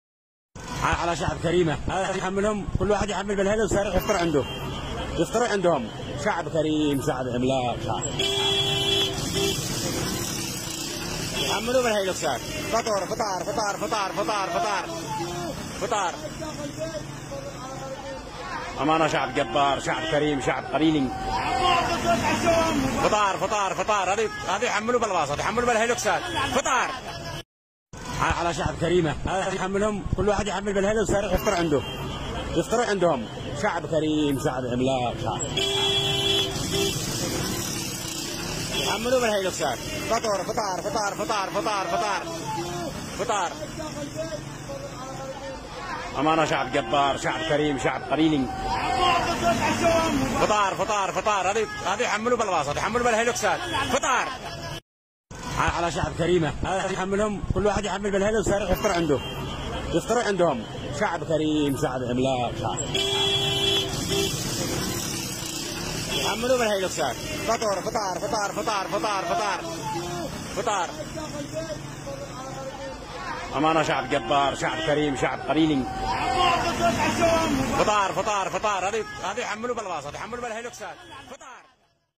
المدينة الوحيده في العالم التي يقفل فيها المطاعم في رمضان|مدينة إب في اليمن .
مدينة_إب كرم _ضيافة#بلا_حدود
مدينة إب او محافظة إب اليمنية المدينة الخضراء العاصمة السياحية لها واهلها كرم لايوجد في اي بقاع الارض .انهم في شهر رمضان المبارك يقومون باغلاق جميع المطاعم فيها وذلك لانهم يستضيفون ويطعمون كل الناس في بيوتهم سواء كانو مارين بالشارع او قاصدين مدن اخرى .
انه الكرم العربي الاصيل والذي وصى به ديننا الحنيف .
يخرج اصحاب المنازل لاستضافة قدر كبير من الناس الماره في الطرقات لتناول الافطار وكذلك وجبة العشاء .